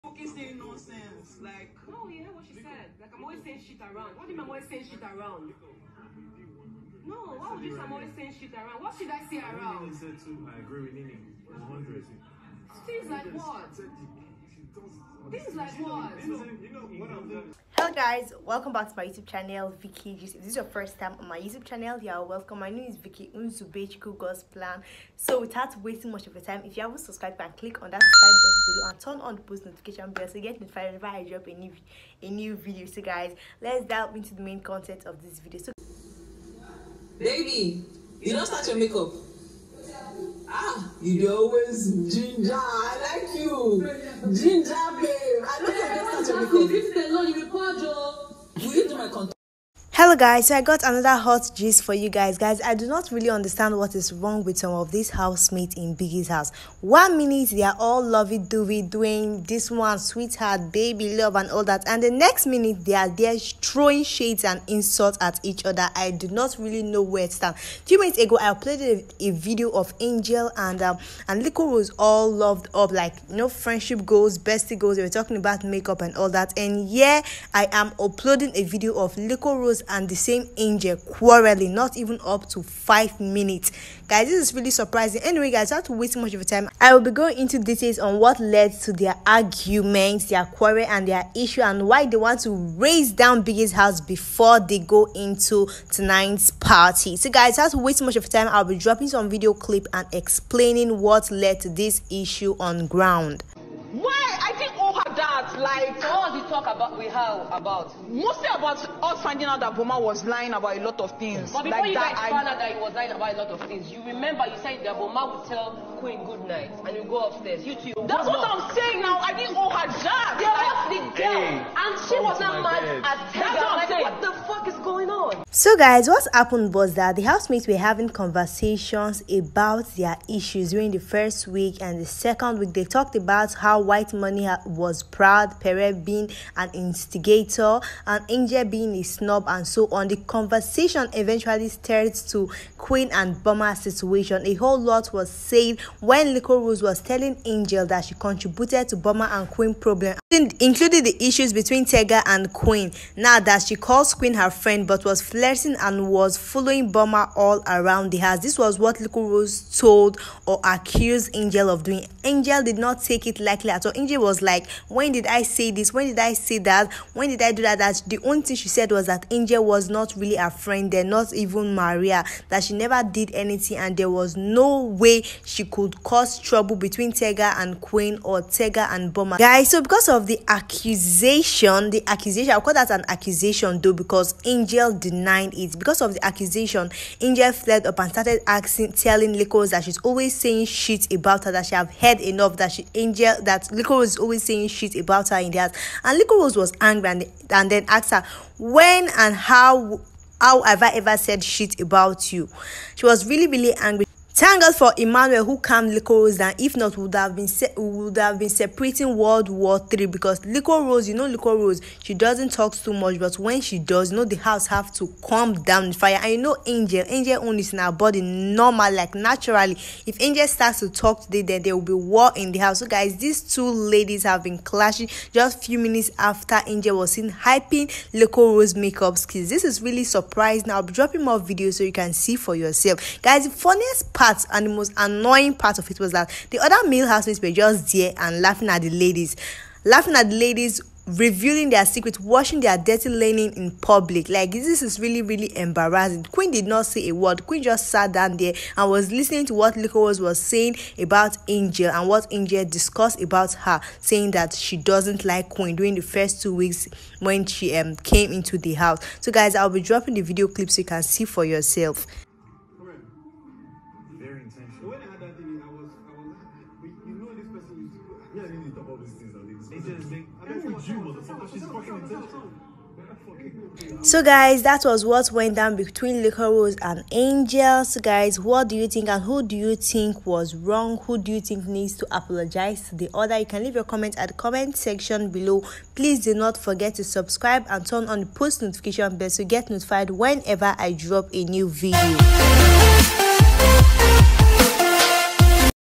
GBAS GBOS 🔥 LIQUOROSE AND ANGEL FIGHTS DIRTY/“WASH THEIR DIRTY LINEN IN PUPLIC”
Angel and Liquorose quarel